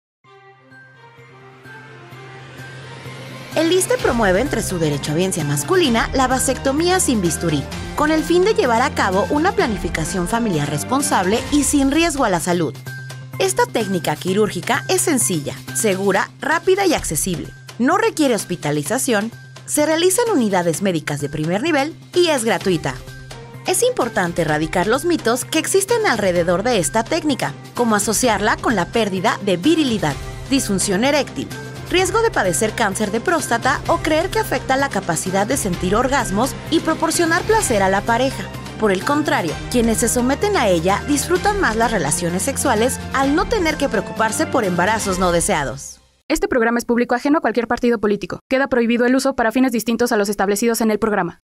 Vasectomía sin bisturí contribuye a planificar familias sin riesgos a la salud
Se promueve entre la derechohabiencia y toda la población masculina el método de la vasectomía sin bisturí para la planificación familiar responsable y sin riesgo a la salud.